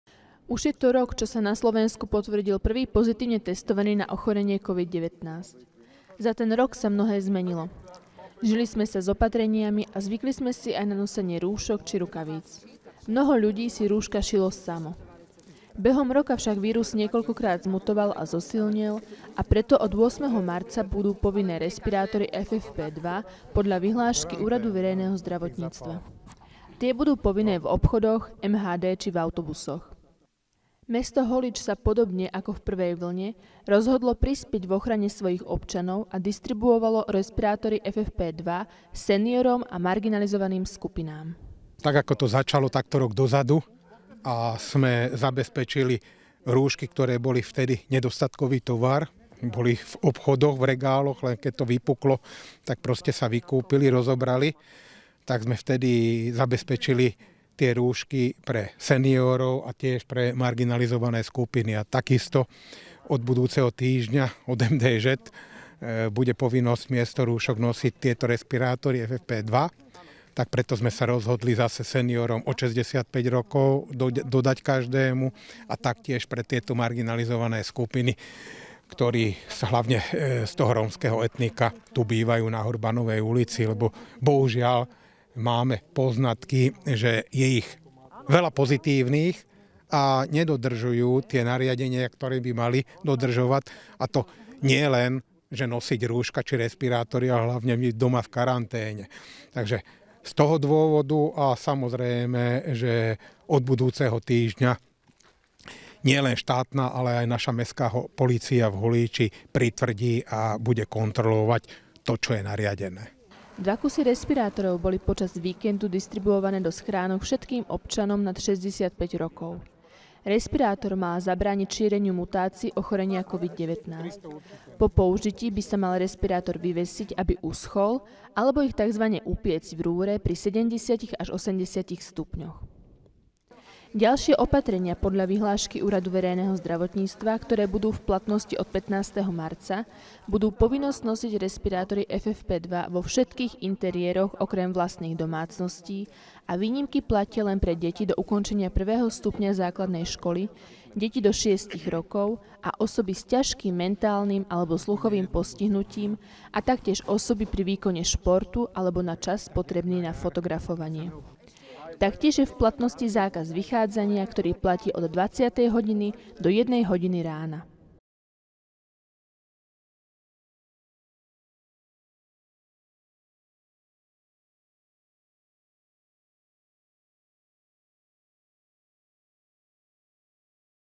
Nové opatrenia
Od 3. marca platia na celom Slovensku nové opatrenia proti šíreniu koronavírusu, najmä čo sa týka zákazu vychádzania. Zároveň je povinnosťou od 8. marca nosiť ochranné respirátory FFP2 v obchodoch a hromadnej doprave. Mesto Holíč sa rozhodlo distribuovať tieto respirátory občanom nad 65 rokov a marginalizovaným skupinám obyvateľov. Zároveň bude na dodržiavanie opatrení v meste dohliadať mestská a štátna polícia